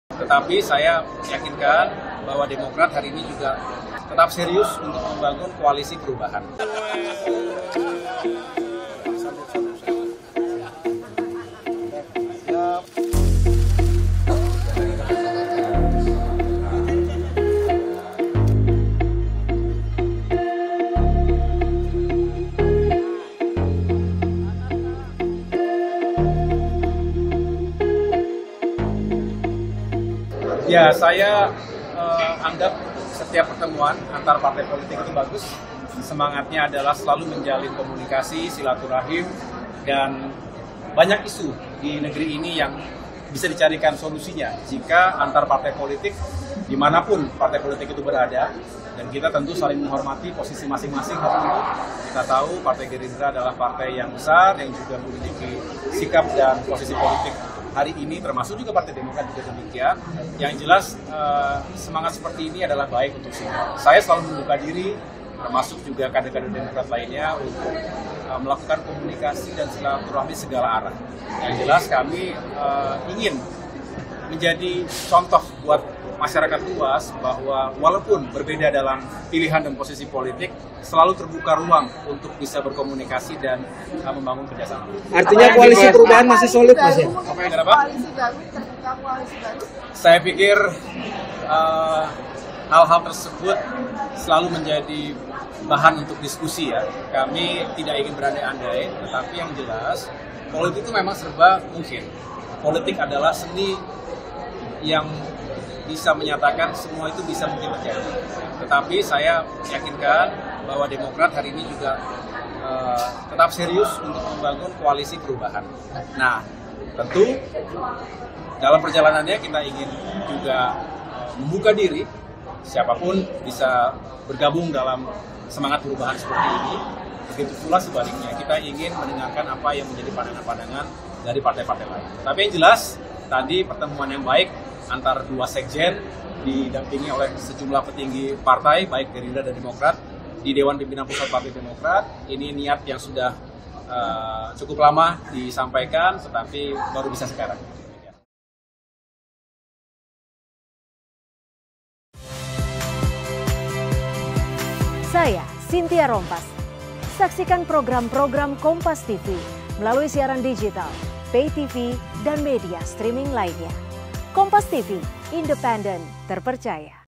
Tanggapan AHY Soal Pertemuan Sekjen Demokrat dan Gerindra
SLEMAN, KOMPAS.TV - Ketua Umum Partai Demokrat Agus Harimurti Yudhoyono (AHY) menegaskan partainya masih berkomitmen menjaga keutuhan Koalisi Perubahan untuk Persatuan dengan mengusung Anies Baswedan sebagai bakal calon presiden.

Hal ini diutarakan AHY menanggapi pertemuan antara Sekjen Partai Demokrat dan Sekjen Partai Gerindra di Kantor DPP Partai Demokrat, Jakarta, pada Kamis (20/7/2023) siang

"Politik itu serba mungkin, politik adalah seni yang menyatakan semua itu bisa mungkin terjadi. Tetapi saya yakinkan bahwa Demokrat hari ini juga tetap serius untuk membangun koalisi perubahan," ujar AHY saat ditemui di UGM, Yogyakarta, Kamis (20/7/2023).

Menurutnya, pertemuan antara pengurus partai politik adalah bagian dari silahturahmi sekaligus memberikan contok kepada rakyat bahwa meski berbeda posisi dan pilihan politik, namun selalu terbuka ruang untuk bekerjasama.

"Saya selalu membuka diri begitu juga kader-kader Demokrat lainnya untuk melakukan komunikasi dan silaturahmi segala arah, yang jelas kami ingin menjadi contoh buat masyarakat," ucapnya.